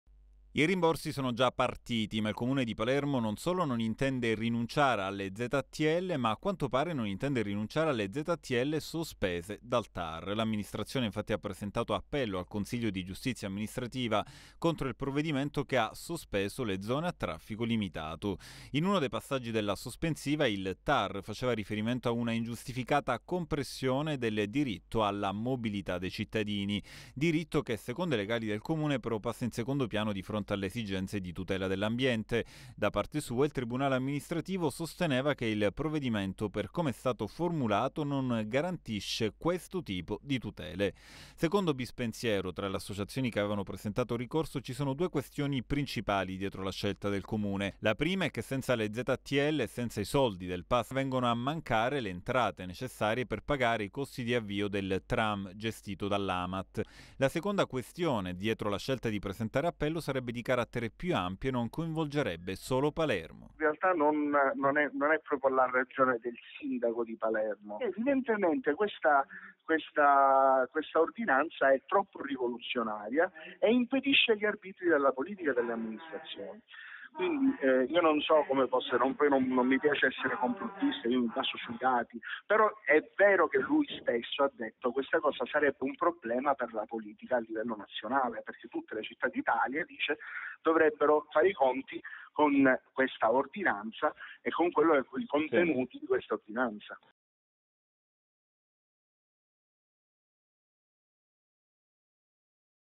Ztl, Comune decide di presentare appello al Cga [TgMed 03/04/2016]
Mentre continuano i rimborsi dei pass, si
scrive un'altra pagina della vicenda delle Ztl a
Palermo. Il Comune ha infatti deciso di
presentare ricorso contro la sospensiva del Tar.
LUCA SALERNO.